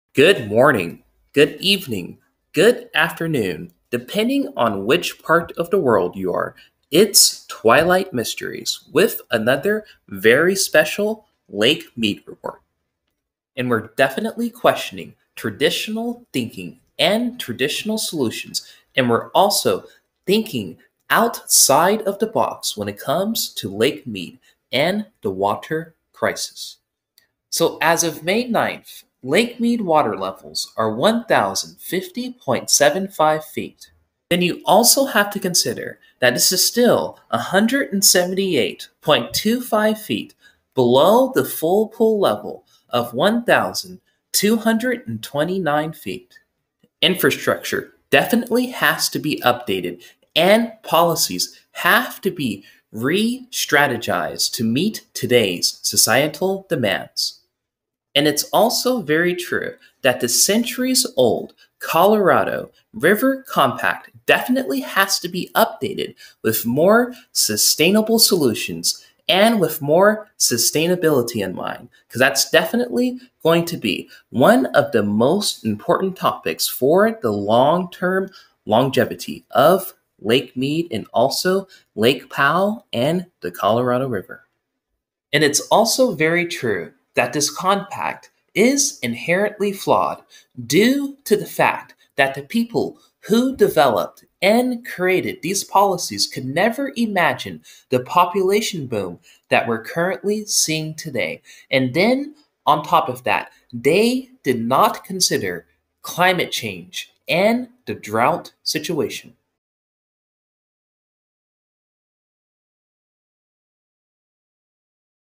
Breaking: Lake Mead Water Levels Soar as Snow Melts!
Lake Mead water levels have reached 1050 feet, as heat waves are expected to melt, massive amounts of snow, in the upcoming months. The Colorado River Compact and its, outdated policies have logical flaws, for today's societal demands. No one back in 1922 could ever conceptualize, the population boom, were seeing today.